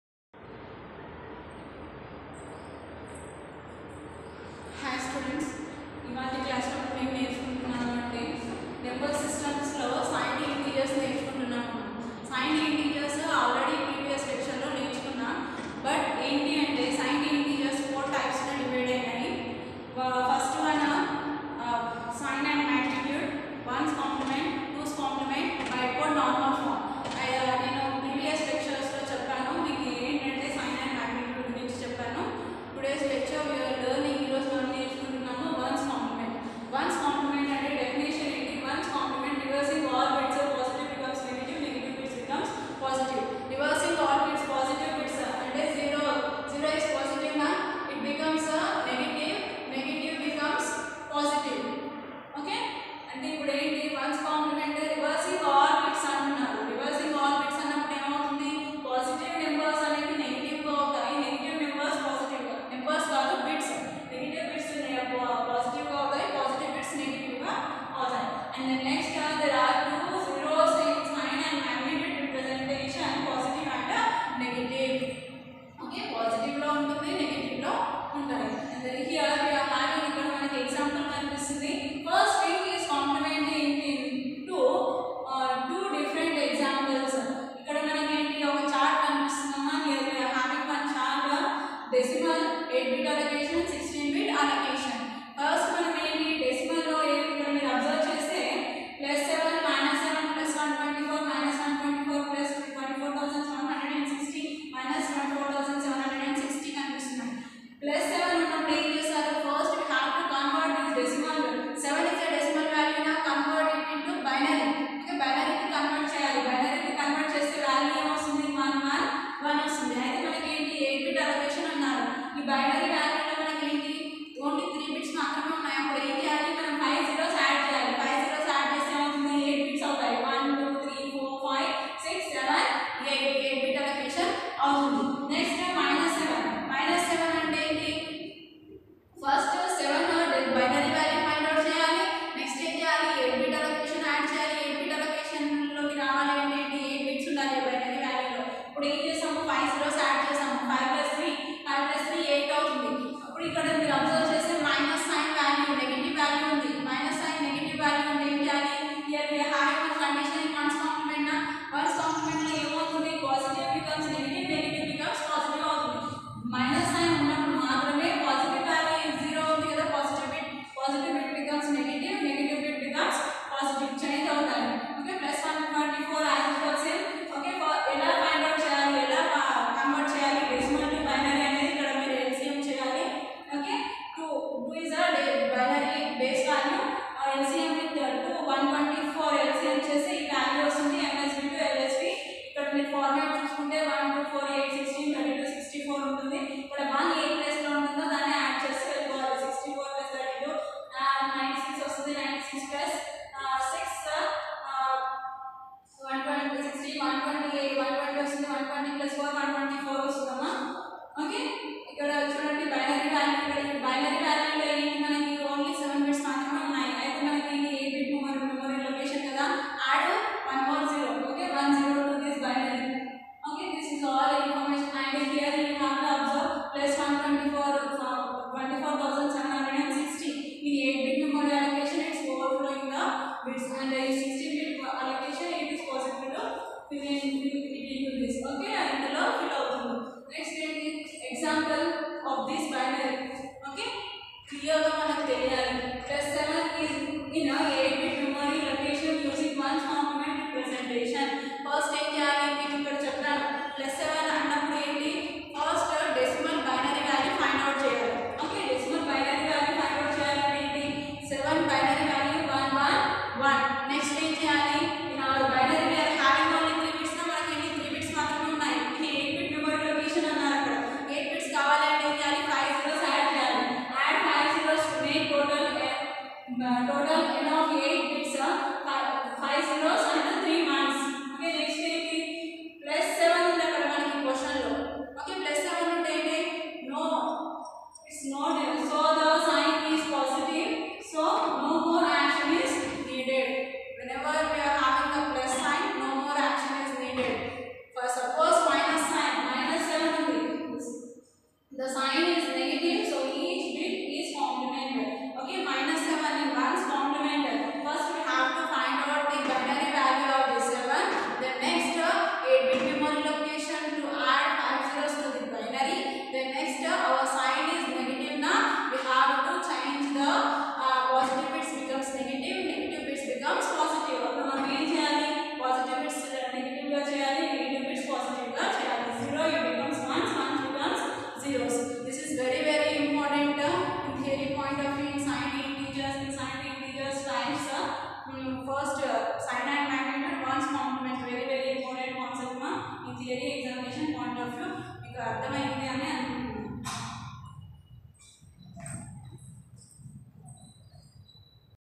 FIT l Lect - 20 TM l One's Complement -Fundamentals of Information Technology
Fundamentals of Information Technology tutorial Discussed One's Complement